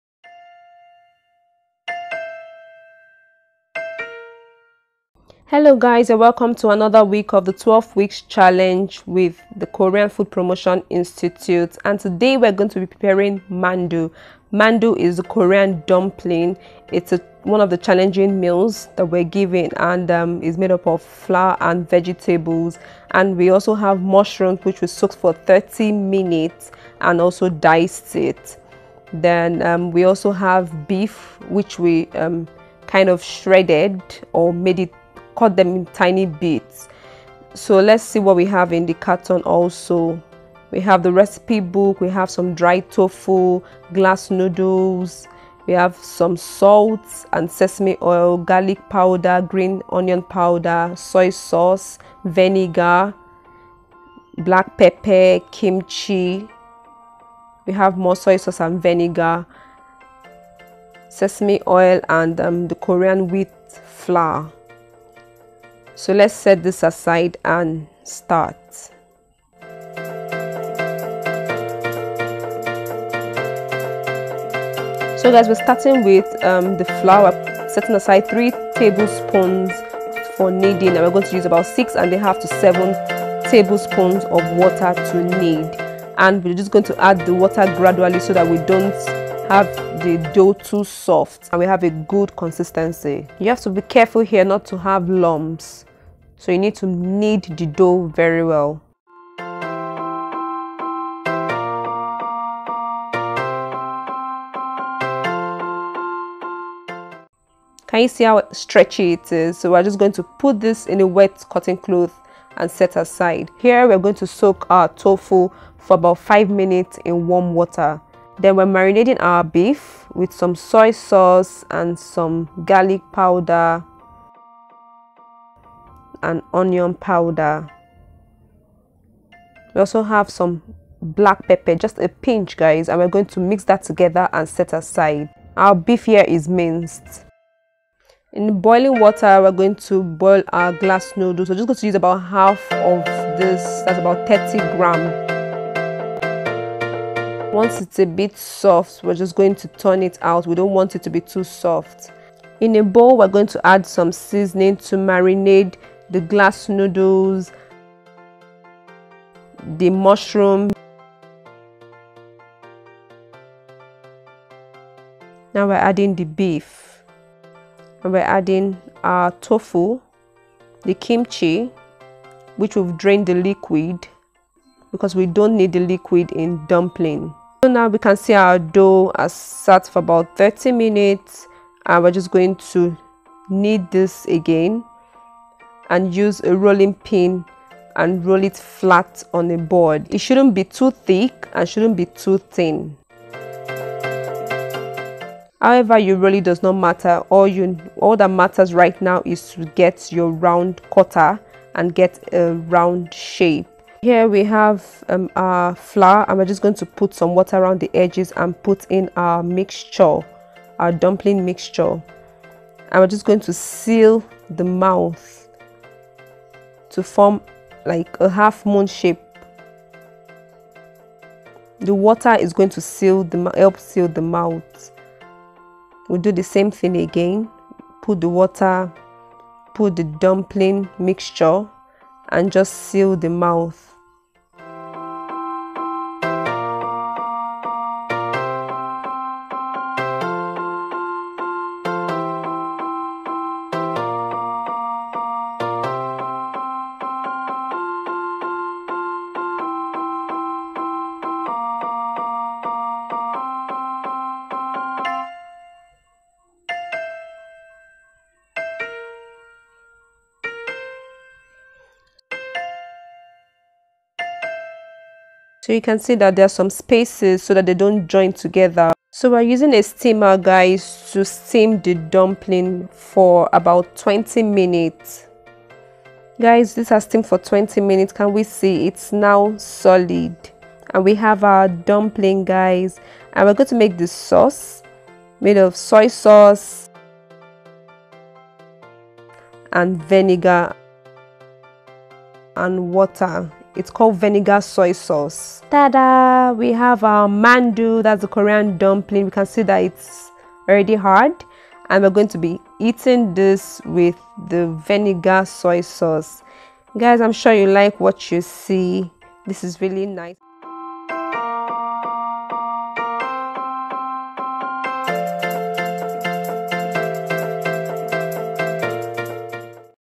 How to make the Korean Mandu || 만두
Mandu are dumplings in Korean cuisine. Mandu can be steamed, boiled, pan-fried, or deep-fried depending on your preference.

 This Mandu was prepared with fillings such as ground beef, tofu, kimchi, potato noodles, etc.